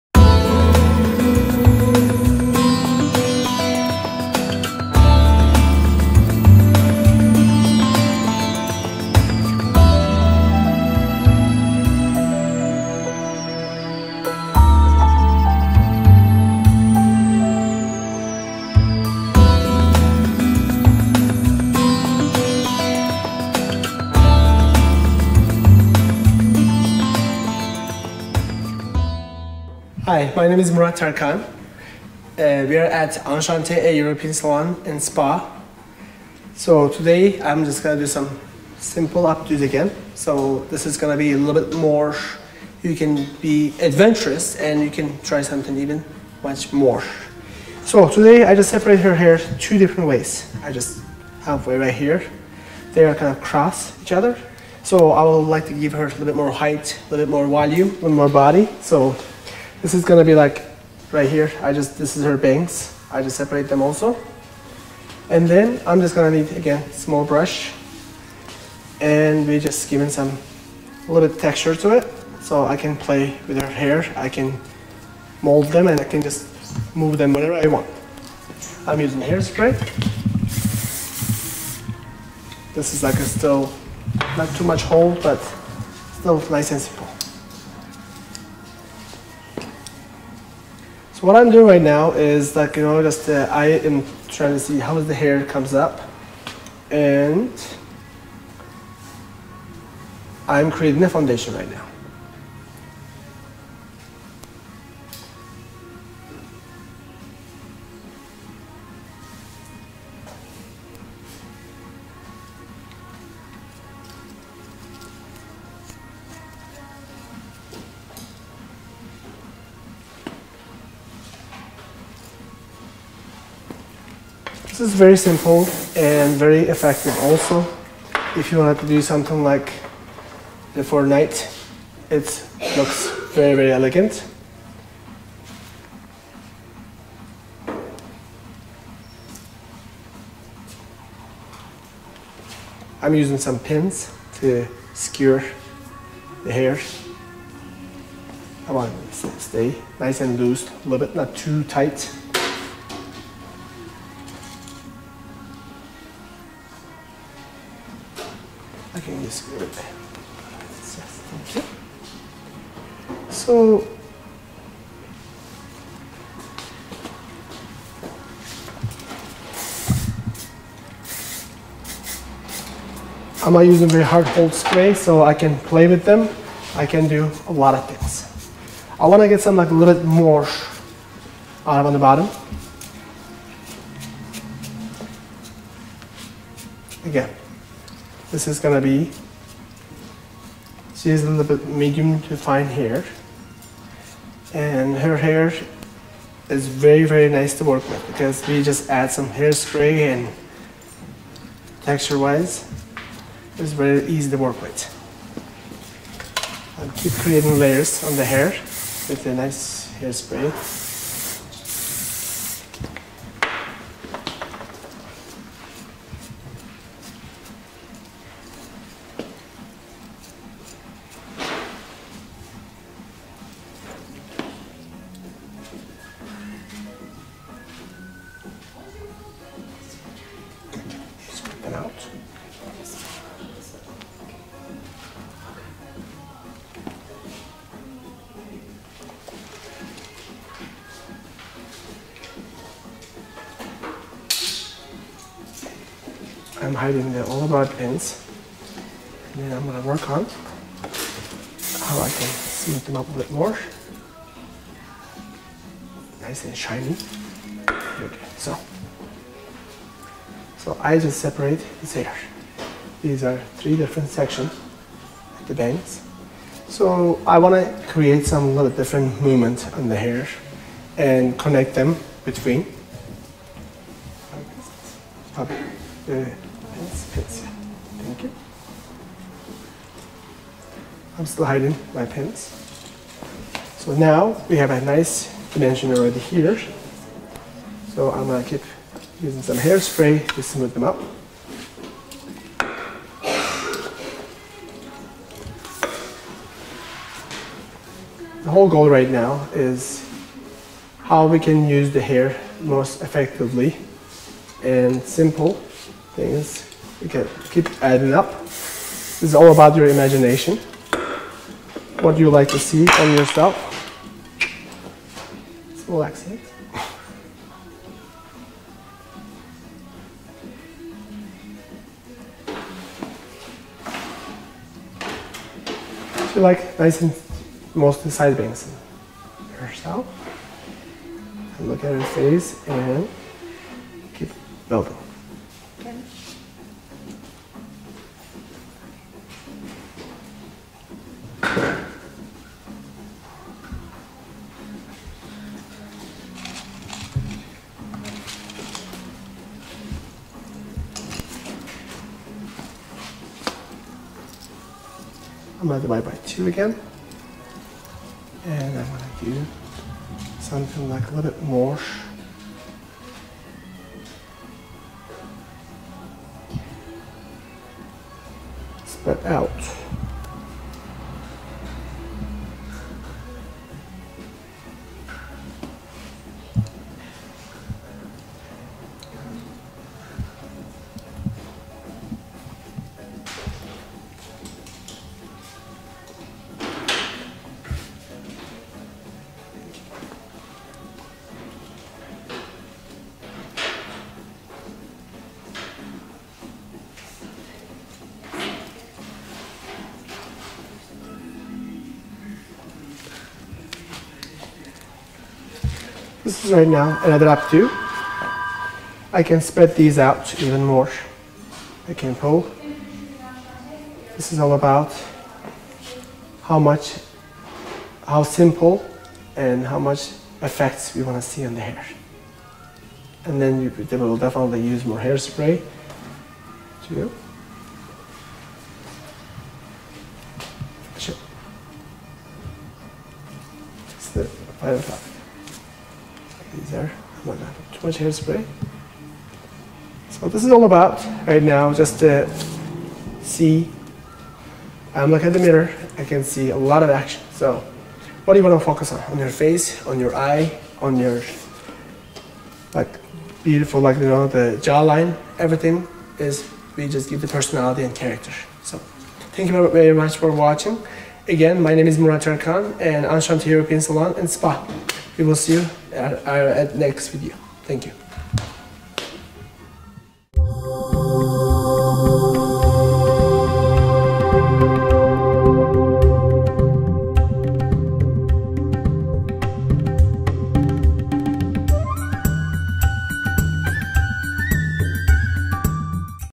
Best easy 5 minute Blow out, Updo's, STYLES by Enchante A European Salon in Fort Collins CO
Murat Tercan, Executive Director at Enchante A European Salon & Spa , shows tips for creating quick and easy (yet elegant) Long Hair Updos curls for the clients on the go beautiful night out. You dressed up so beautifully but hair its adds up more to your look or taking away. So try at home to how can you create these styles. Come see us our New Salon in Fort Collins location.